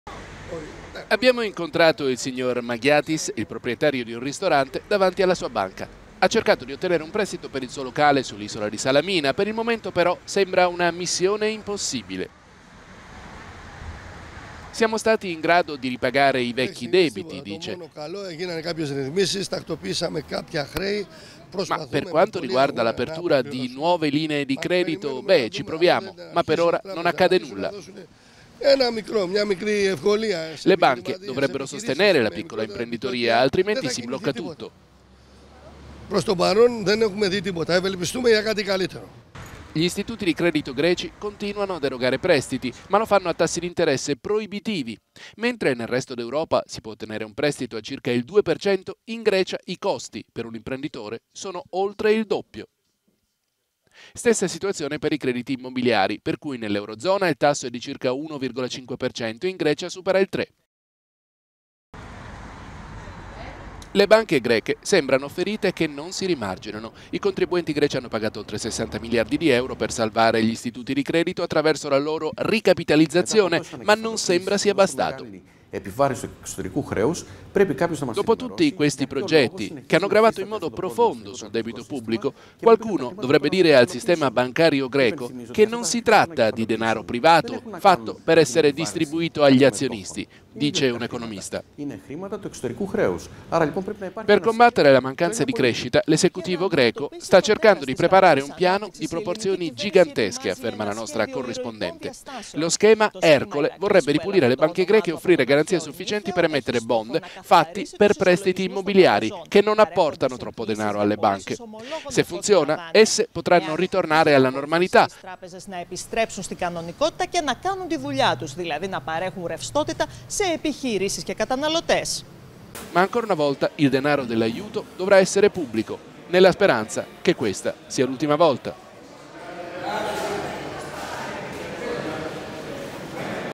Il "vizietto" delle banche greche
Gli istituti di credito greci non prestano soldi agli imprenditori malgrado i miliardi erogati dai piani di salvataggio…